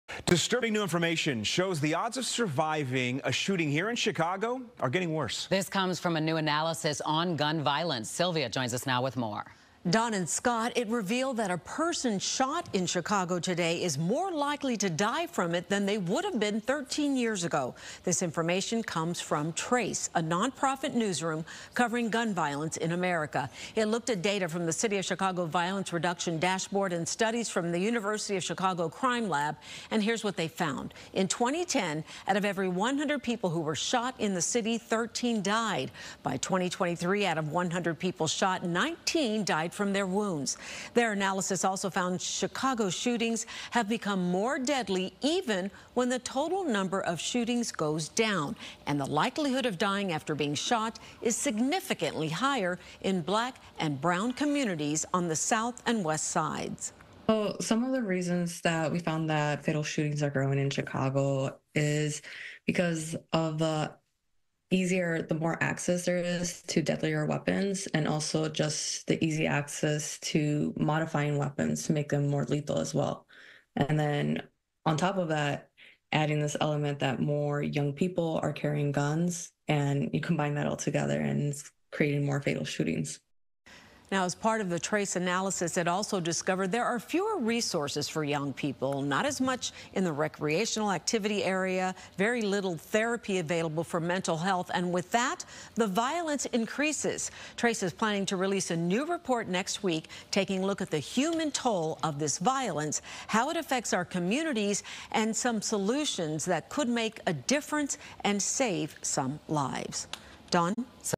Chicago gun violence becoming deadlier despite fewer shootings, analysis finds
A recent analysis of gun violence in Chicago has revealed a disturbing trend: the likelihood of surviving a shooting has decreased over the past 13 years.